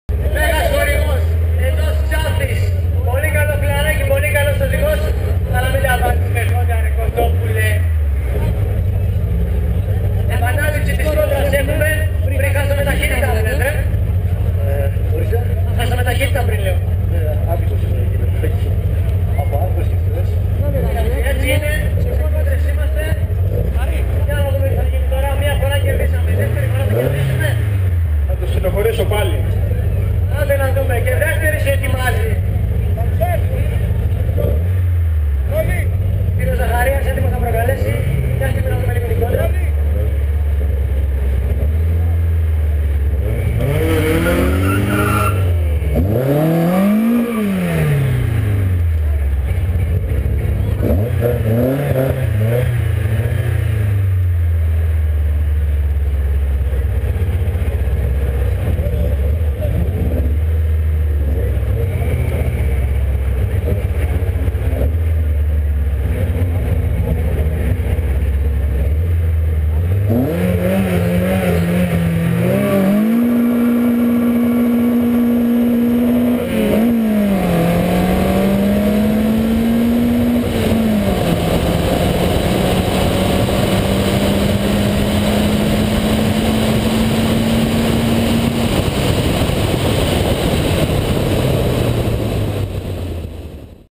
TETRAKOSARI III - VW Golf II onboard (Karali) vs Citroen Saxo (Theodoropoulos)
Από την δεύτερη ημέρα (Κυριακή) του ΤΕΤΡΑΚΟΣΑΡΙ ΙΙΙ, όπου εκτός από τη διαδικασία Elimination και φυσικά την απονομή, έγιναν πολλές προκλήσεις από τους οδηγούς μέσω μικροφώνου!

Το ΤΕΤΡΑΚΟΣΑΡΙ ΙΙΙ στέφθηκε με απόλυτη επιτυχία! Το περιοδικό Αυτοκίνηση Αν. Μακεδονίας & Θράκης, για άλλη μια φορά κατάφερε να αποδείξει πως μόνο αυτοί που πραγματικά αγαπούν τον μηχανοκίνητο αθλητισμό μπορούν να κάνουν τόσα πολλά μέσα σε τόσο μικρό χρονικό διάστημα. Γραφτείτε στο forum του TETRAKOSARI.GR τώρα, και σχολιάστε το διήμερο και όλες τις κόντρες που είδατε!
Διαβάστε περισσότερα στο τεύχος 13!

Διοργάνωση:
Περιοδικό Αυτοκίνηση Αν. Μακεδονίας & Θράκης